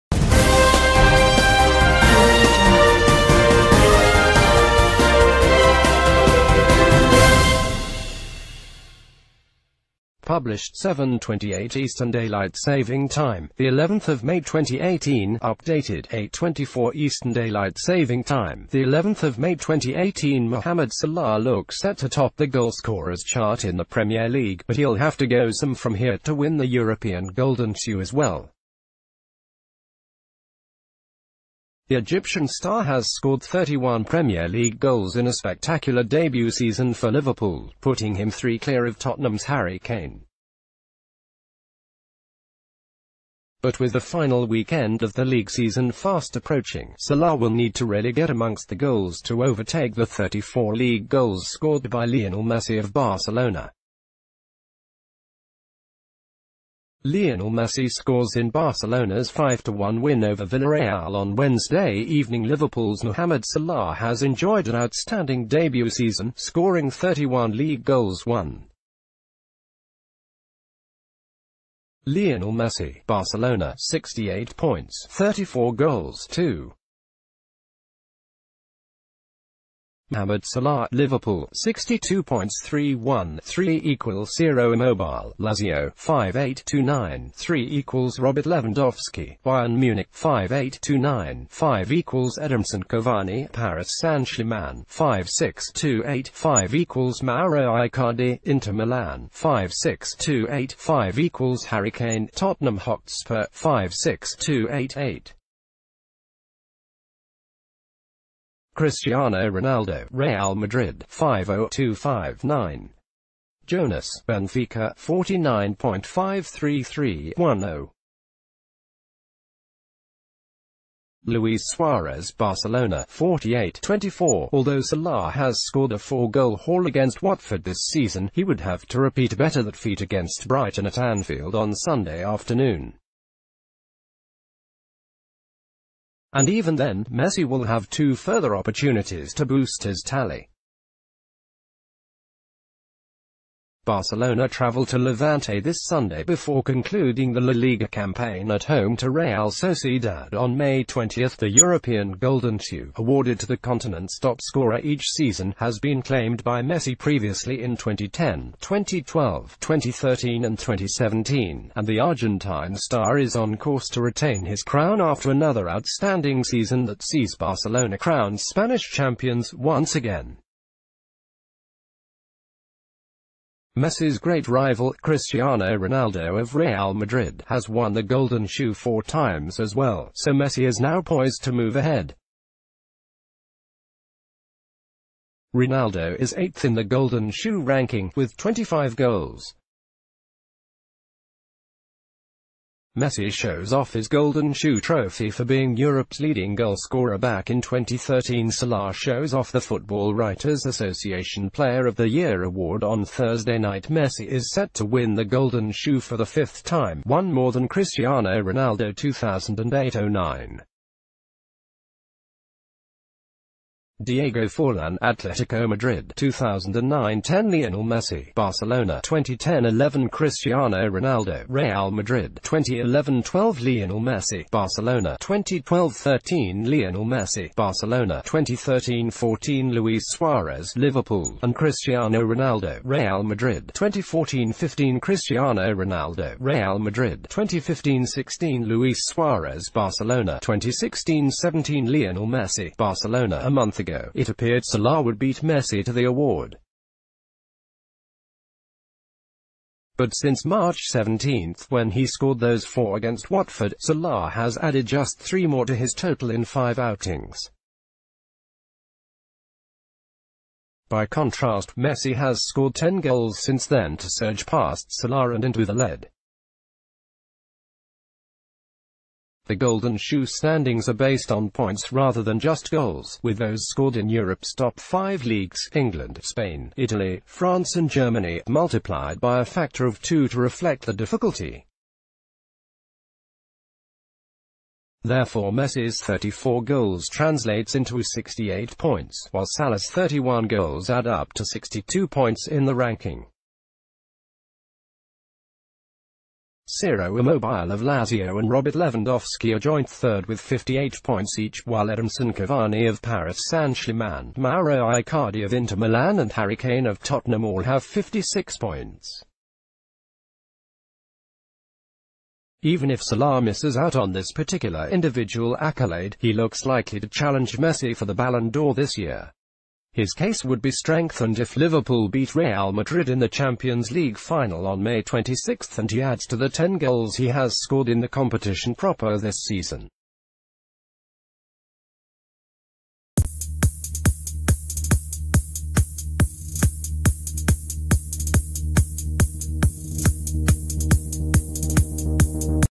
Breaking News 24H -Mohamed Salah will be defeated by Lionel Messi at Golden Golden Shoe
Published: 07:28 EDT, 11 May 2018 | Updated: 08:24 EDT, 11 May 2018 Mohamed Salah looks set to top t...